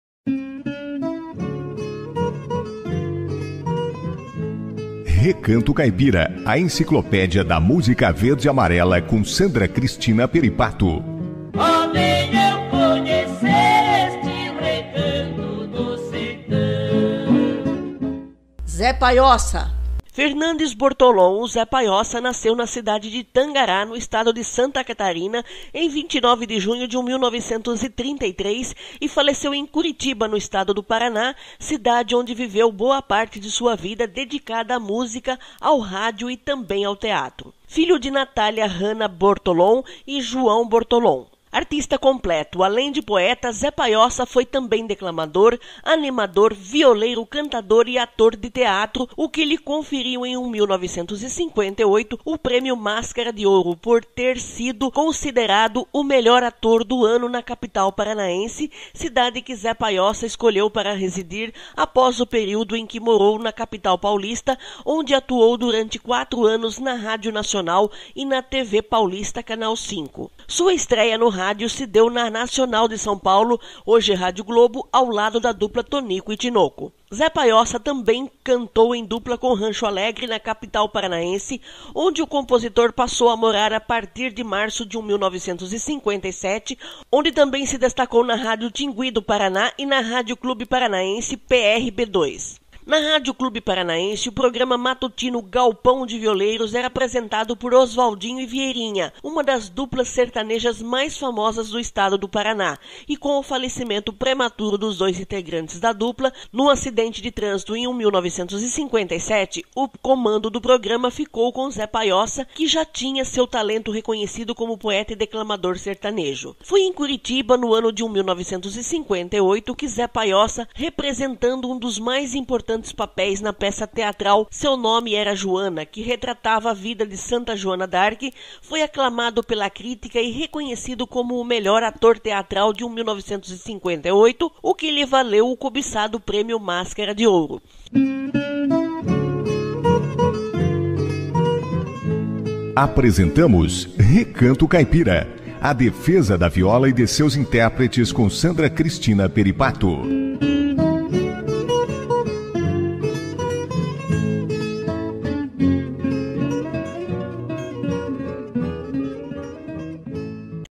Recanto Caipira com Sandra Cristina Peripato - Biografia de Zé Paioça
Quadro Recanto Caipira
Sandra Cristina Peripato narra a Biografia de Zé Paioça
Texto: Sandra Cristina Peripato